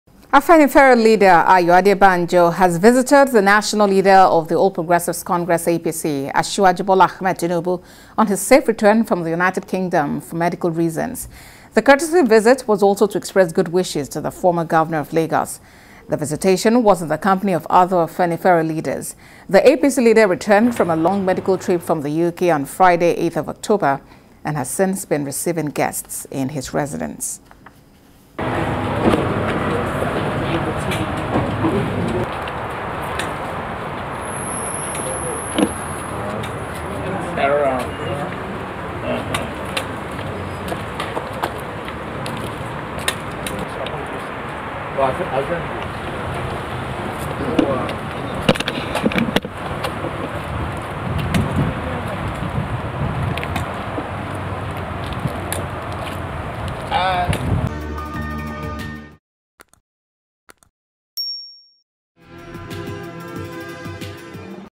Ayo Adebanjo, Other Afenifere Leaders Visit Bola Tinubu In Lagos
Afenifere leader, Ayo Adebanjo has visited the National leader of the APC Asiwaju Bola Tinubu on his safe return from the United Kingdom for medical reasons.

iPhone: Go to apple store newspaper review